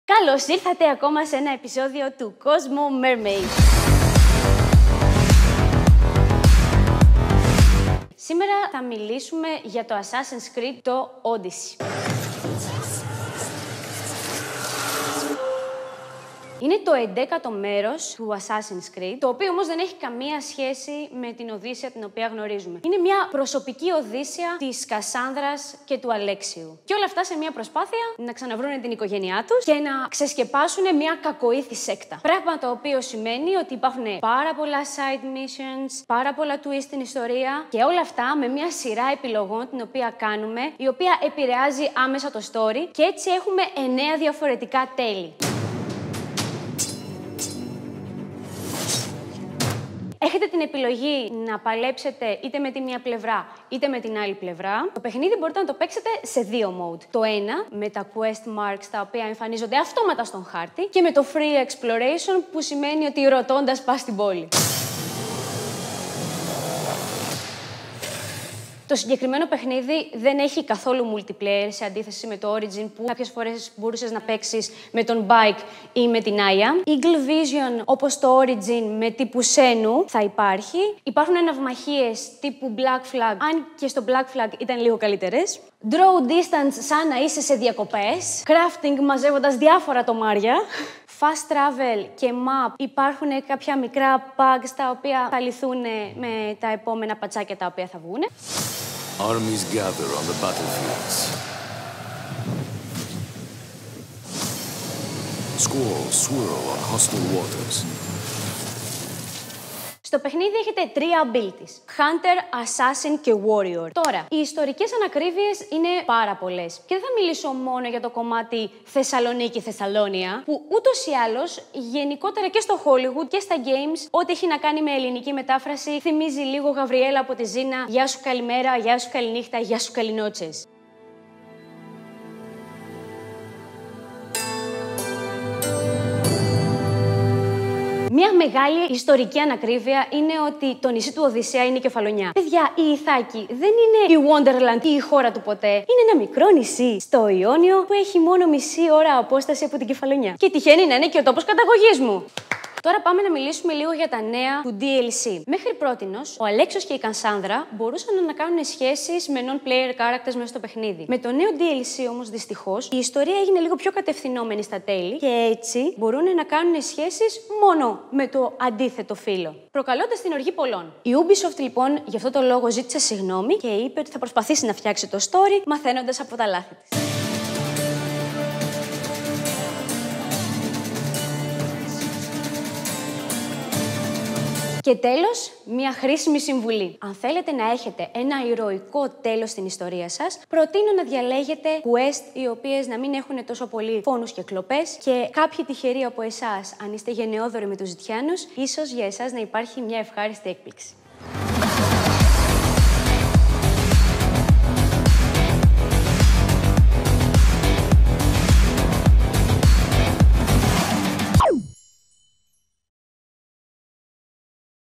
CosmoMermaid επ. 2: Assassin's Creed Odyssey
Η Βασιλική Βλασσοπούλου σε αυτό το επεισόδιο του CosmoMermaid, μιλά για το Assassin's Creed Odyssey.